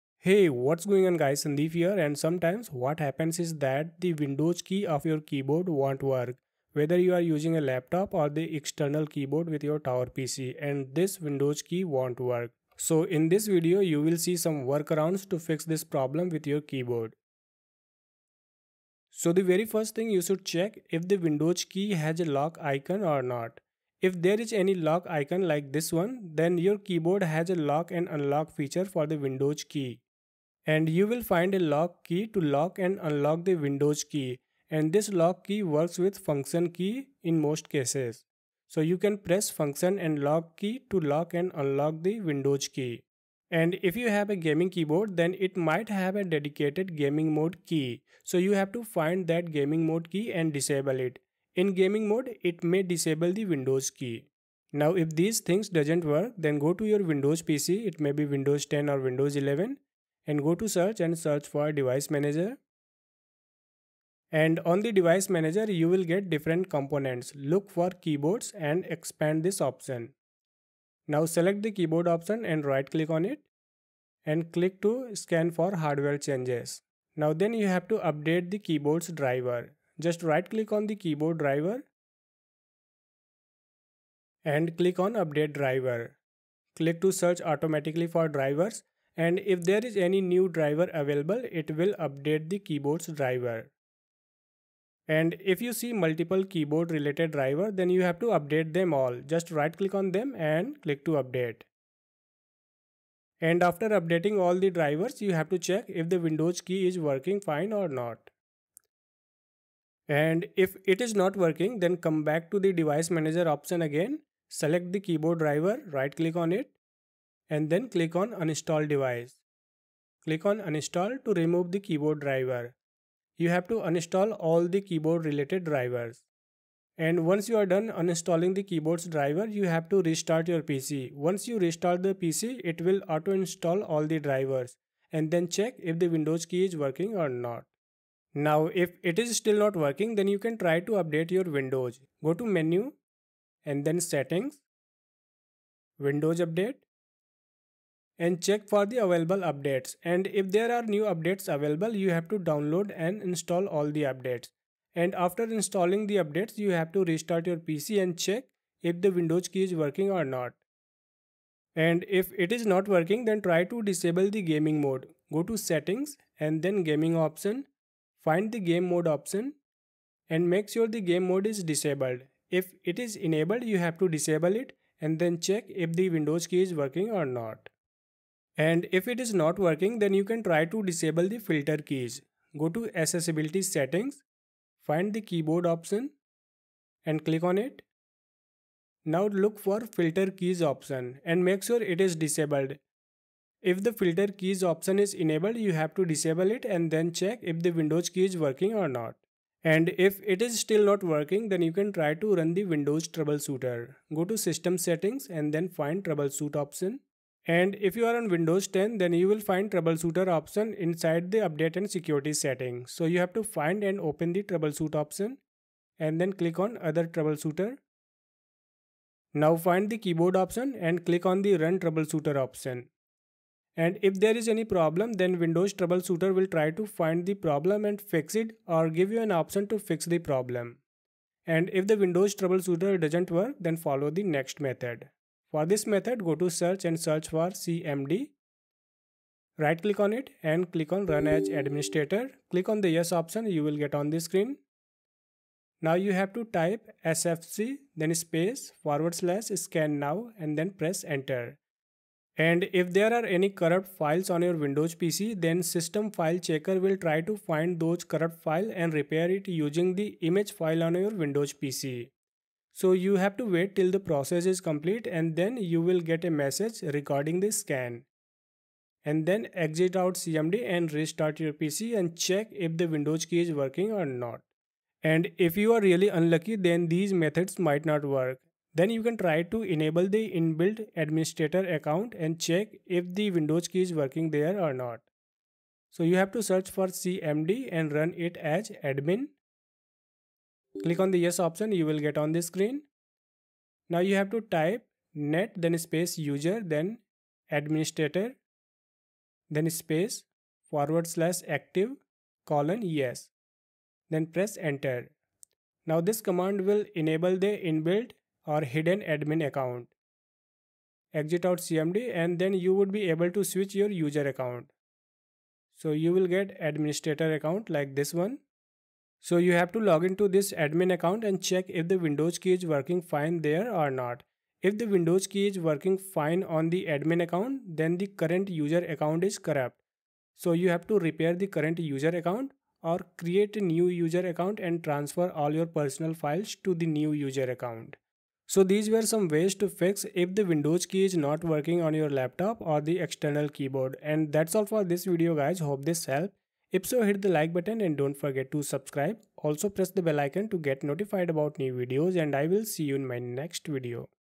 Fix Windows Key Not Working On Keyboard With Windows 11/10 PC | Fix Start Button
Why is my windows key not working? There could be different problems with the keyboard or Windows 10 or 11. You need to find and fix the Windows key not working on the laptop issue. In this video, you will find different ways to fix the problem.
You might have a laptop or an external keyboard; depending on the issue, any of these methods should help you to fix the underlying problem.

If might get this issue while playing games due to gaming mode or due to Windows issues.

00:00 Intro
00:18 Check for Windows lock key
00:55 Update/Uninstall Keyboard Driver
02:18 Windows Update
02:41 Disable Gaming Mode
02:58 Disable Filter Keys
03:19 Run Windows Troubleshooter
03:52 Repair Corrupt Files
04:38 Create New User Account
05:38 Outro

The above methods should help to fix Start Button on your keyboard.

Any data, IP, emails, and files shown in videos are for demonstration purposes only and do not belong to any third person or entity.